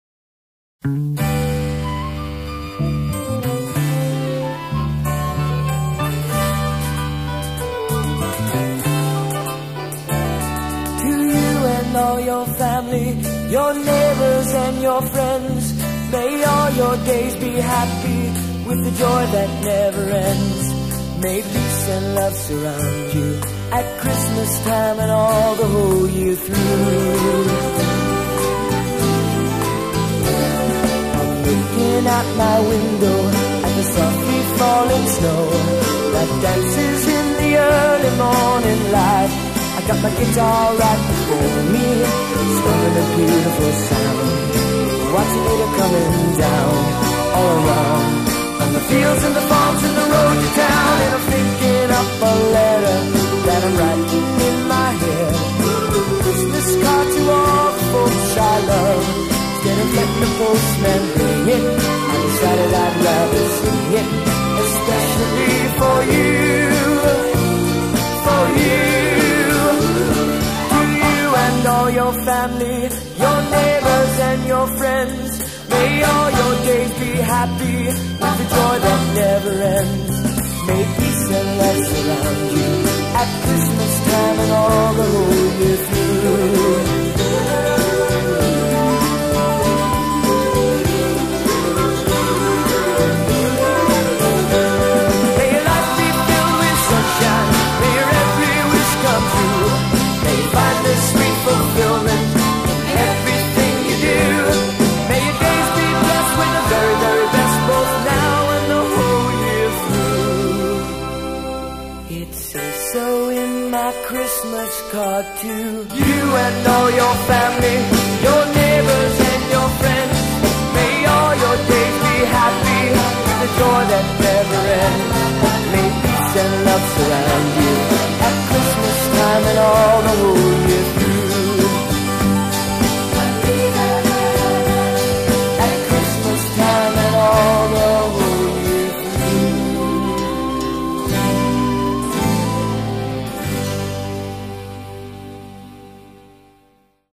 My Christmas Card to You
My christmas wish for everyone using a PartridgeFamily song